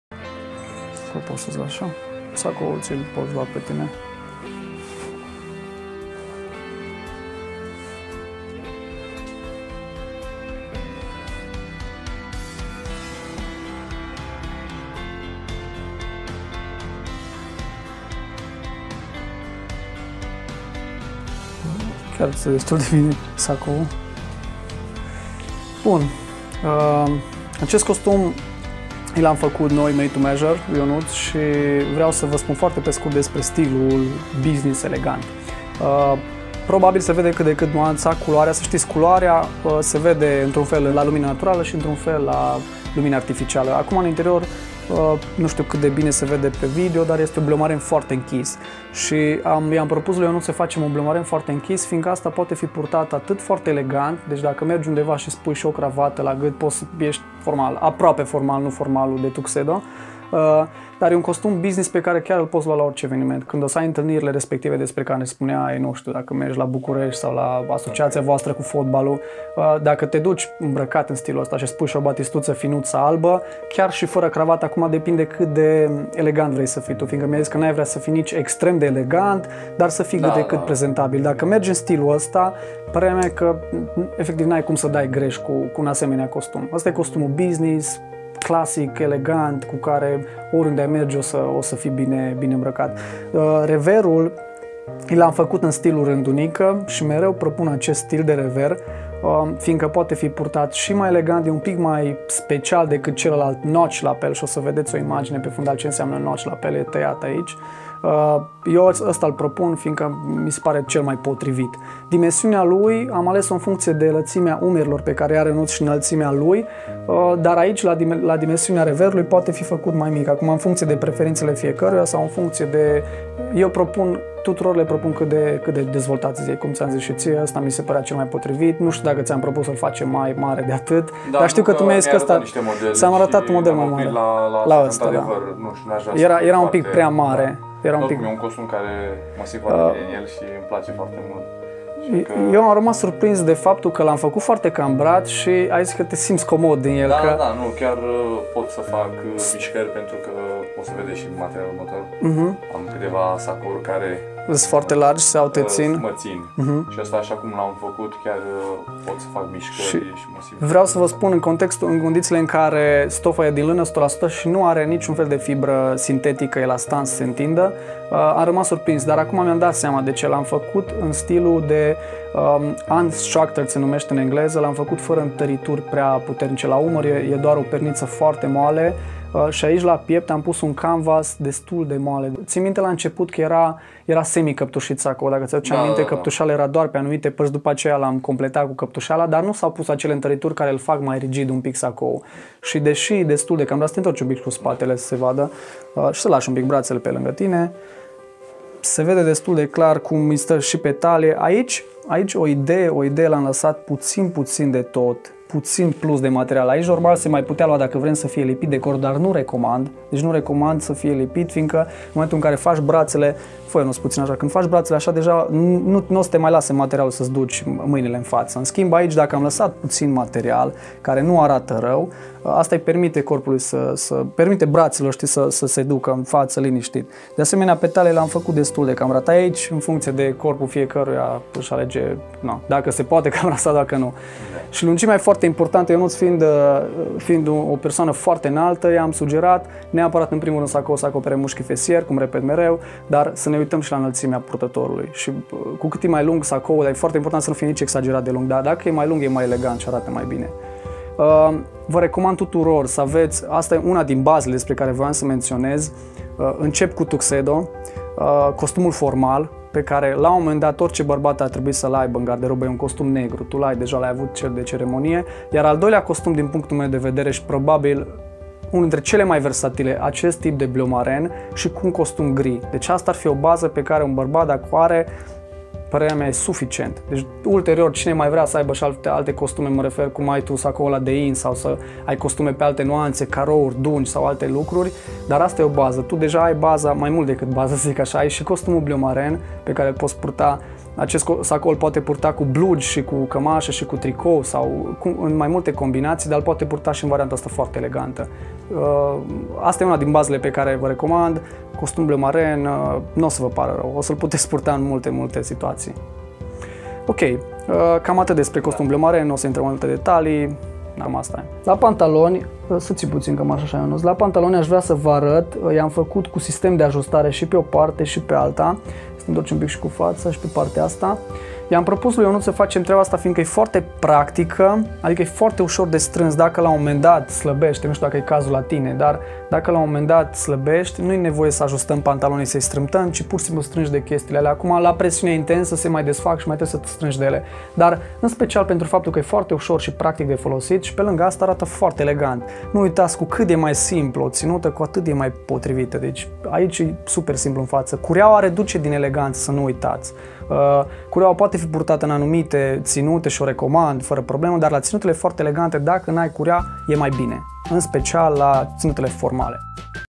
Costumul Bleumarin - Made to Measure
În acest video am prezentat în linii generale unul dintre costumele de bază pe care un bărbat ar trebui să le aibă la un moment dat.

Este vorba despre costumul bleumarin.
Din punctul meu de vedere este în top 3, alături de Tuxedo și costumul gri.

Nuanțele de bleumarin au fost mereu preferatele mele, fiindcă ne permit să purtăm costumul în foarte multe variante( datorită versatilității).

Vă recomand tuturor un costum bleumarin, ideal din fibre naturale.

Am mai menționat despre:

- tipul reverului și cum l-am ales
- Tipul de întăritură interioară
- Cambrarea sacoului
- Lungimea sacoului
- Tipul de pantaloni - sistemul de ajustare vs curea